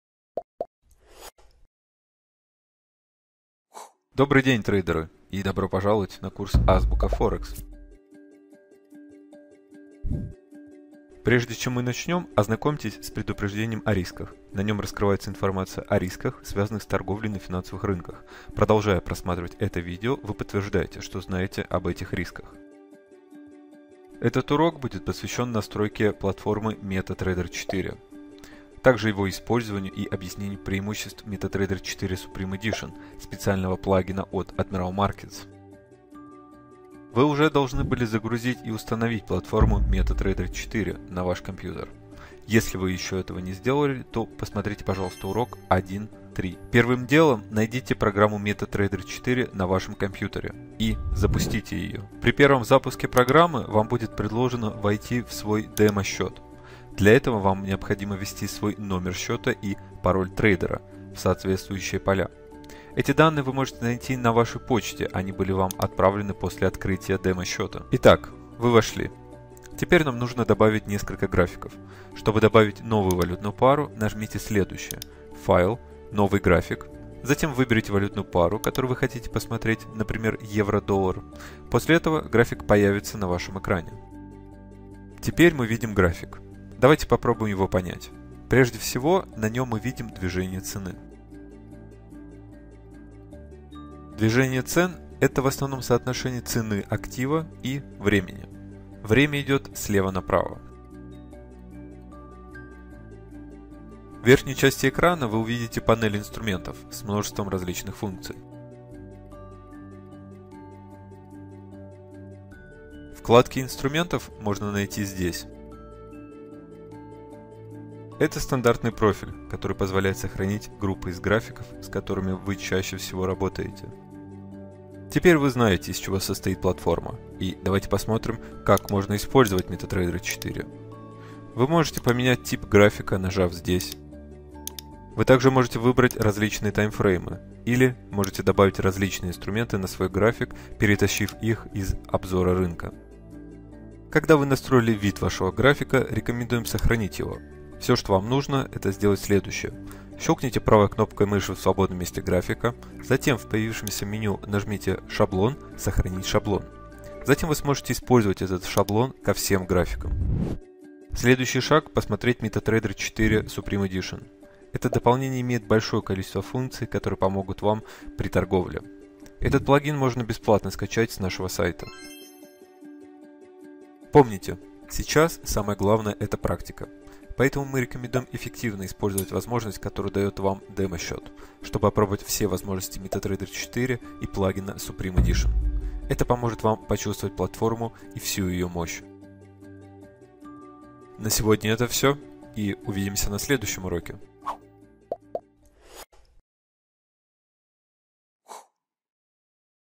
АЗБУКА ФОРЕКС - 2.1 - Настройка MT4
MetaTrader 4 (MT4) является простой платформой со всеобъемлющим доступом и возможностью анализа финансовых рынков. Это видео быстро объясняет, как загрузить и установить программное обеспечение на Ваше устройство, плюс показывает Вам, как настроить основные функциональные возможности платформы.

Как только Вы будете готовы, Вы сможете изучить различные функции платформы, например, открытие финансовых инструментов, проведение анализа, сохранение шаблонов и добавление индикаторов. Кроме того Вы узнаете о преимуществах MT4 Supreme Edition (SE).

В этом уроке Вы узнаете:

1. Как скачать и установить платформу MetaTrader 4. Будут объяснены основные функции торговой платформы.

2. Как использовать платформу MT4 — эта часть показывает, как трейдеры могут получить доступ к торговым графикам внутри платформы. Также будет объяснено, как сохранять шаблоны, изменять таймфреймы и другие важные параметры.

3. Наконец, вы получите информацию о преимуществах плагина MetaTrader 4 Supreme Edition* — это расширение разработано для МТ4 и предлагает трейдерам усовершенствованный способ торговли. Admiral Markets разработали платформу для удовлетворения потребностей своих трейдеров.